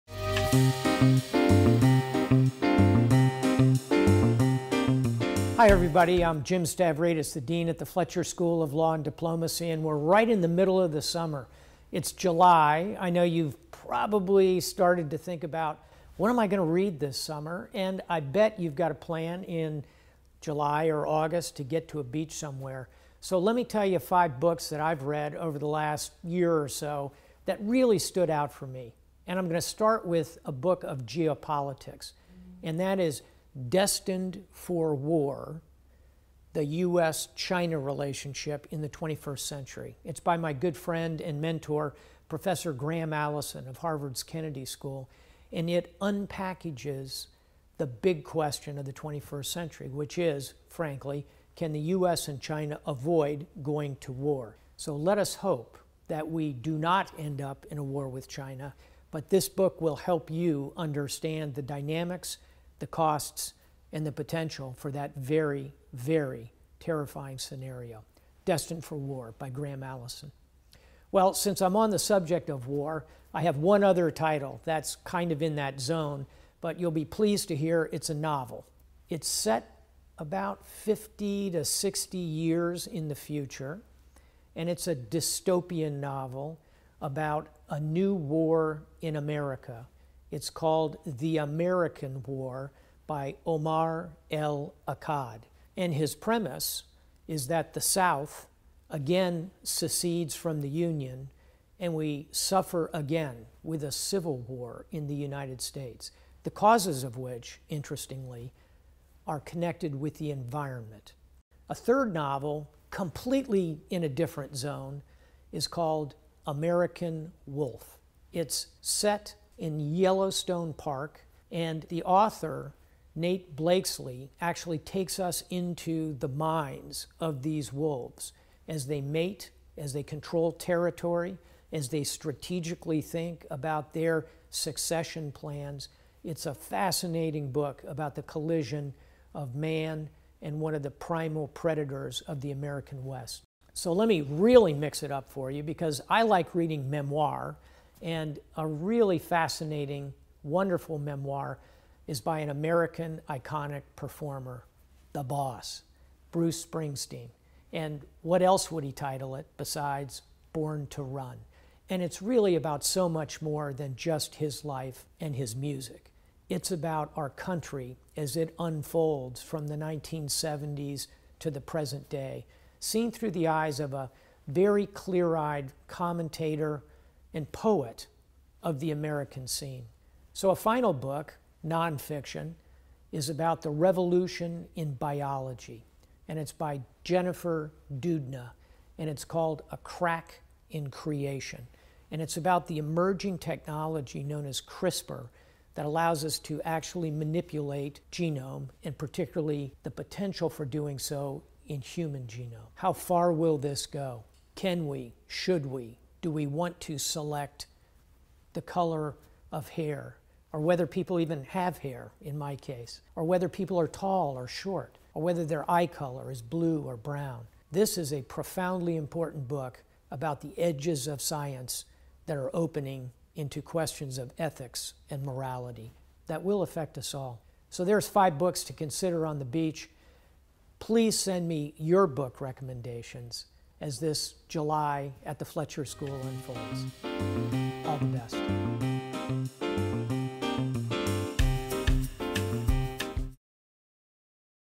Dean Stavridis' Beach Book Recommendations - Summer 2018
Dean James Stavridis shares his recommendations for good books to put in your beach tote as you head off on your holiday. There's something for everyone, with the list featuring an examination of geopolitics, science and human genetics, nature and the wilderness, a dystopian novel and even a memoir by 'The Boss.'